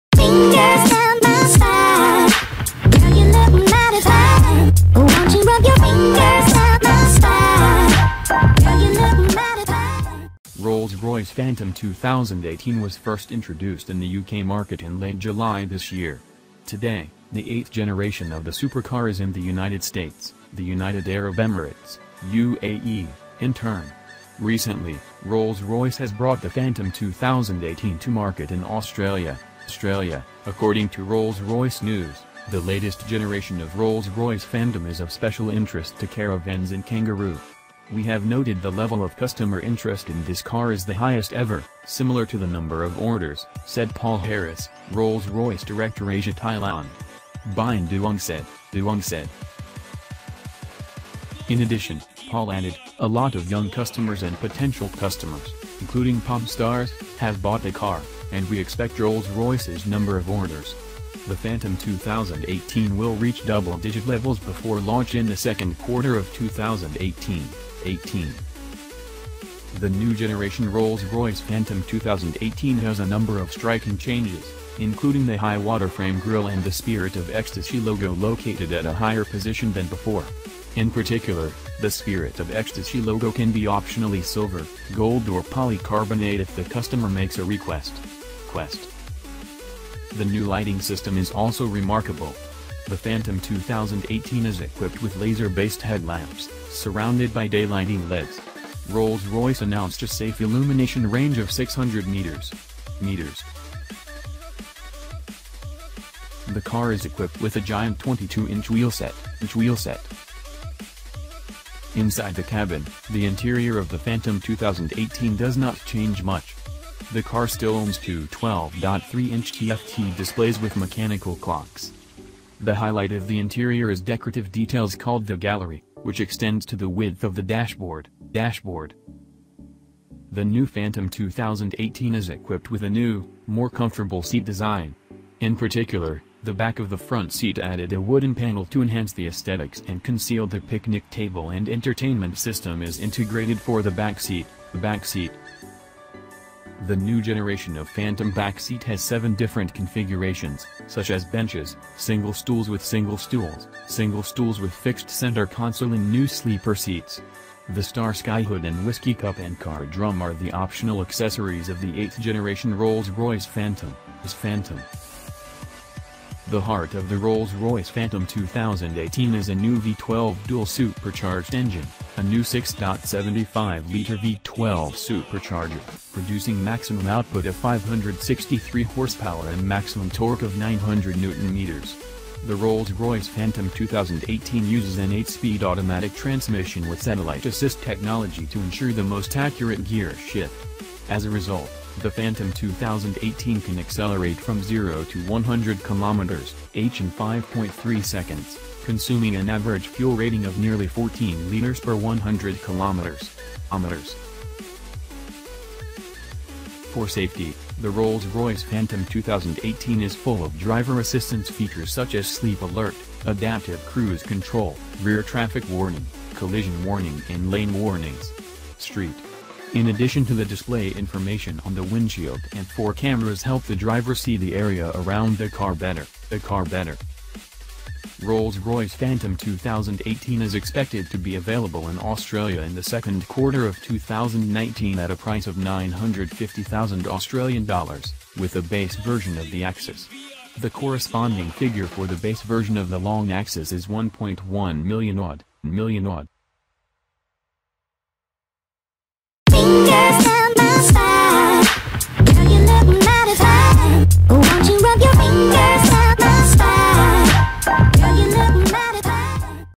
Rolls-Royce Phantom 2018 is sold in Australia | YOURCAR
Rolls-Royce Phantom 2018 is sold in Australia
The Rolls-Royce Phantom 2018 will start shipping in Australia in the second quarter of 2019 at a price of AUD 950,000, for the base version of the Rolls-Royce.